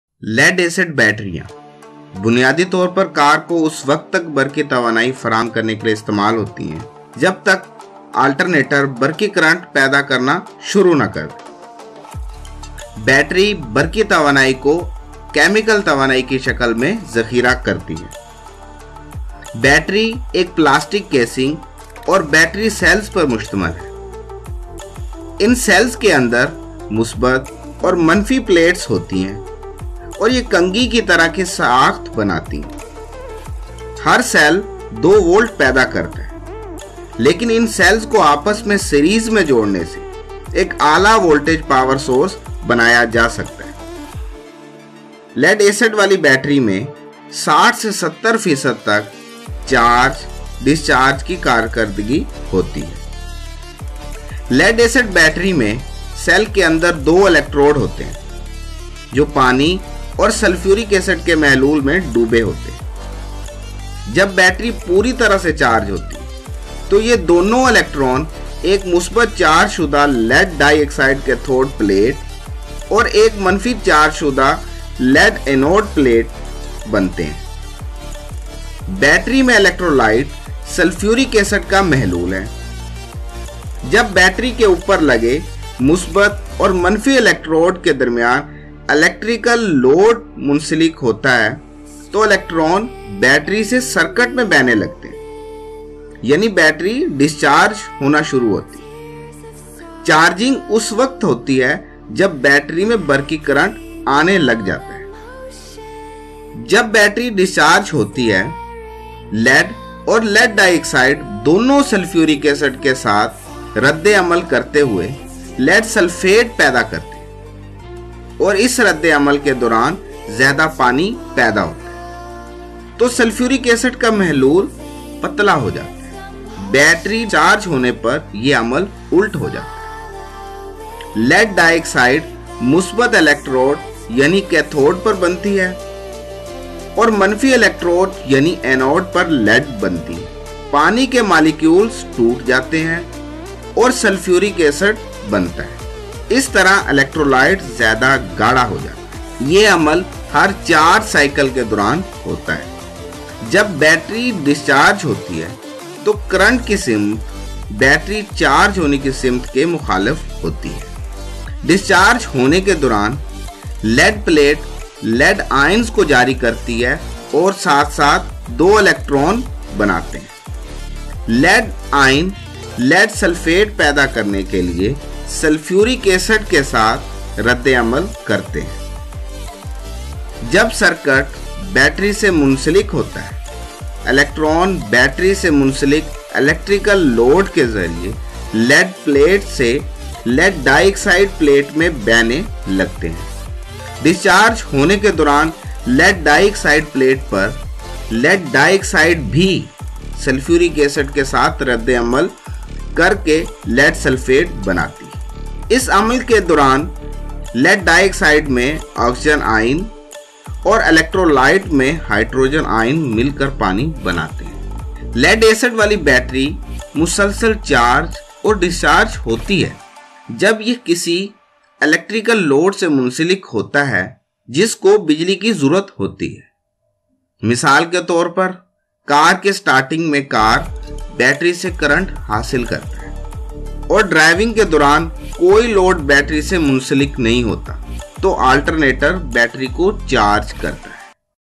How lead acid battery works in Urdu and Hindi
in this video you will learn about How lead acid battery works in Urdu and Hindi.